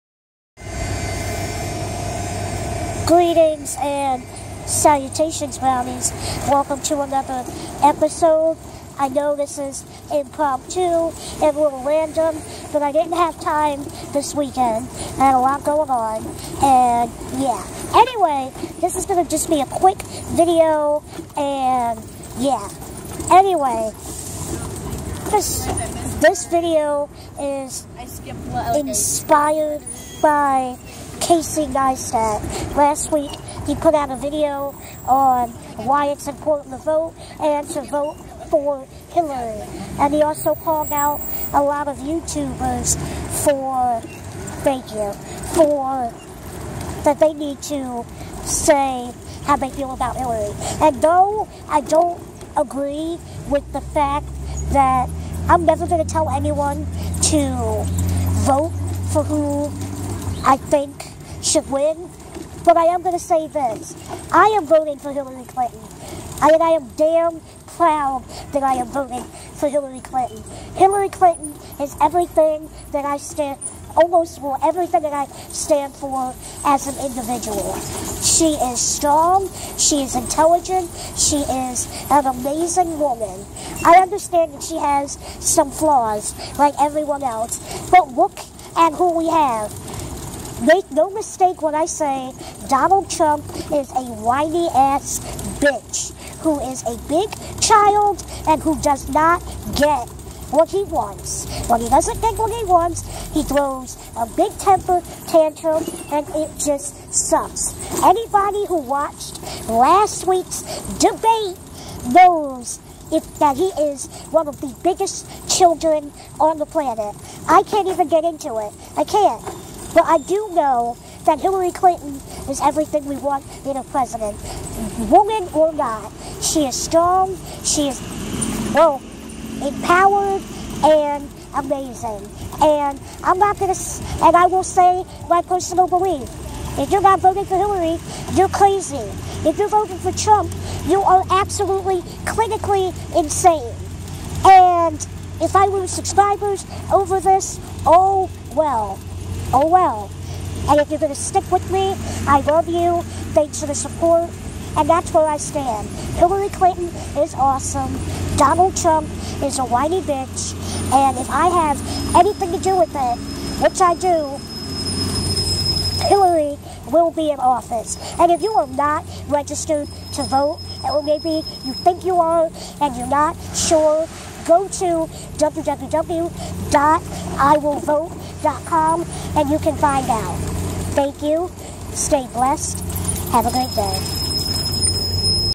Who I am Voting For - Casey Neistat Response
Greetings!
I love making content to make others happy. I post almost daily about anything I want or what my subscribers want.
Greetings! Today I tell you who I am voting for and why? Casey Neistat inspired me!